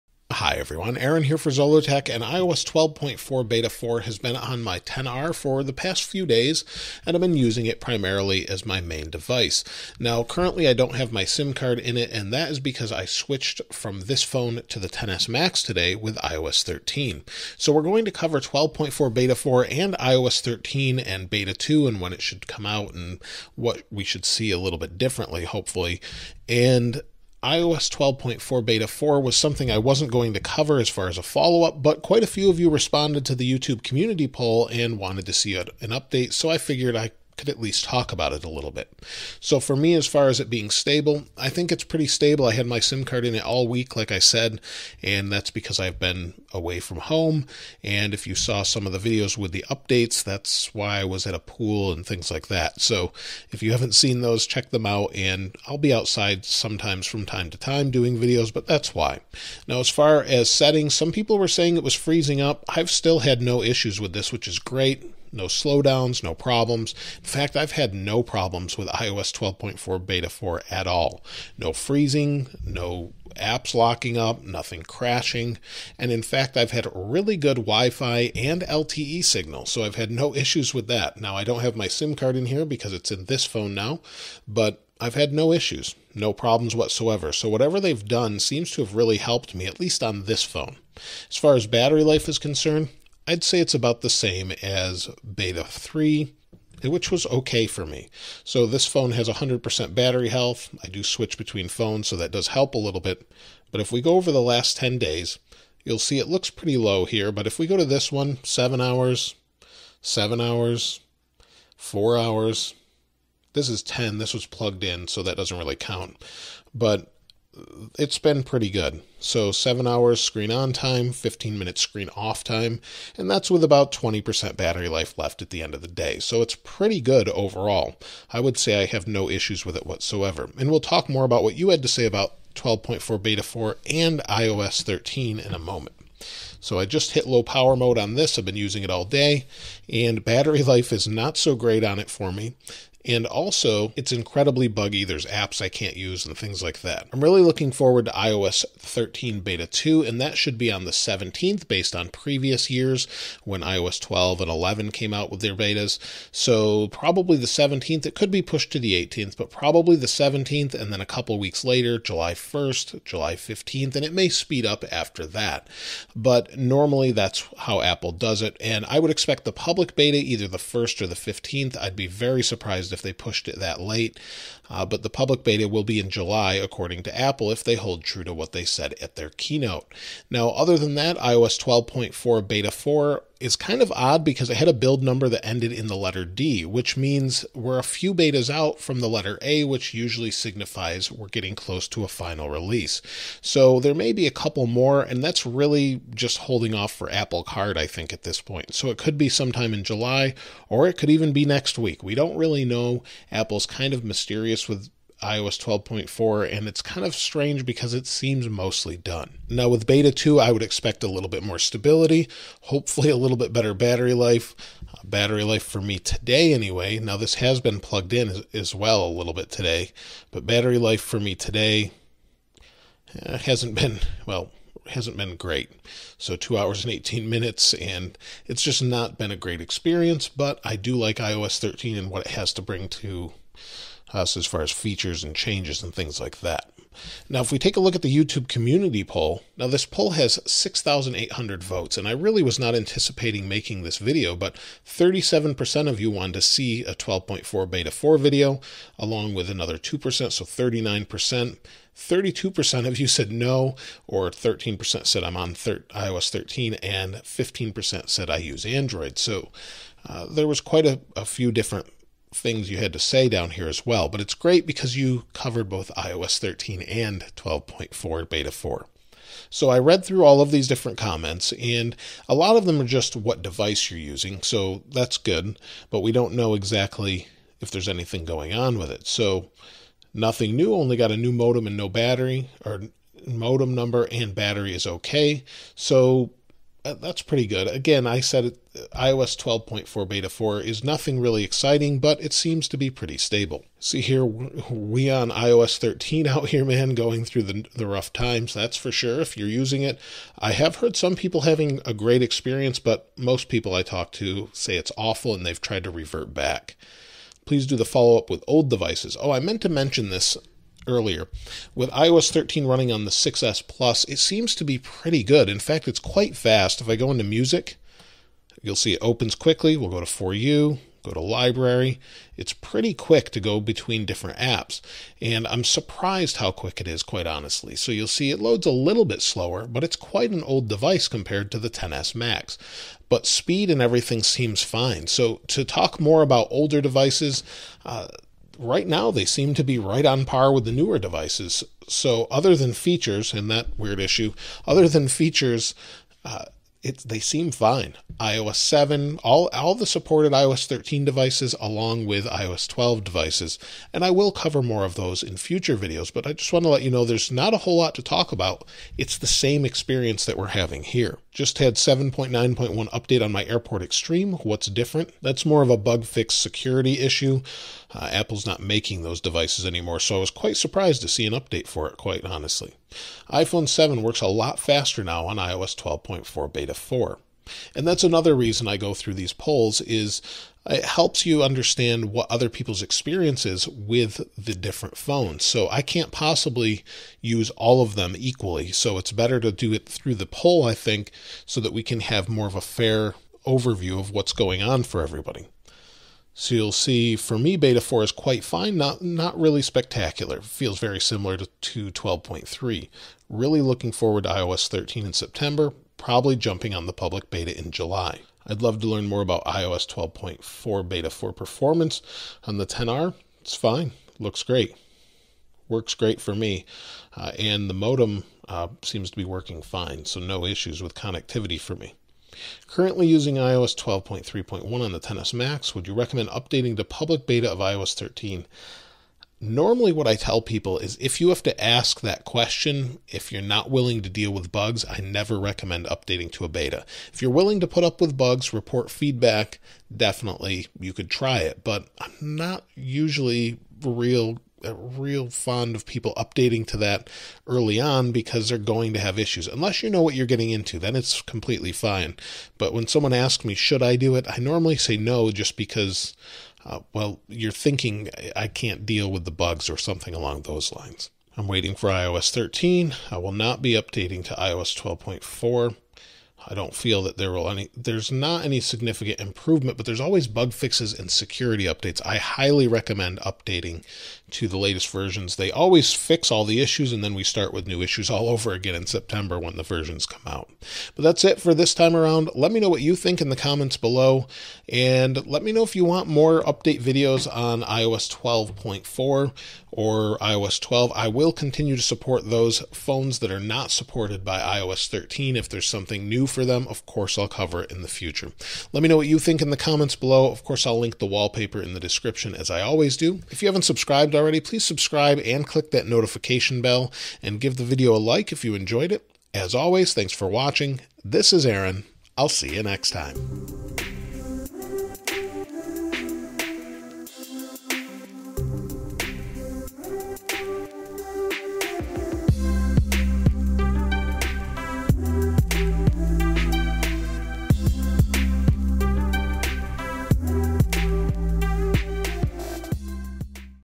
iOS 12.4 Beta 4 and iOS 13 Beta 1 - Follow Up
iOS 12.4 Beta 4 has been out for a few days and iOS 13 Beta 1 has been out for almost two weeks. I share my experience with both iOS 12.4 Beta 4 and iOS 13 Beta 1. iOS 13 has been very buggy with multiple crashes and problems and if you are thinking of installing it on your main devices, I would not just yet. I also go over when I believe iOS 12.4 will come out to the public and when iOS 13 Beta 2 will come out and when IOS 13 Public beta will be out. iphone iOS Apple iOS 13

How has the iOS 13 Beta and iOS 12.4 Beta 4 been for you?  I would love to hear about your experience with iOS 13 and iOS 12.4 Beta 4.

Would you like to see follow-ups on iPad OS as well or would you like them all in one video with iOS 13 videos?

iPhone Case I use for XS Max on Amazon
Check MacBook Prices on Amazon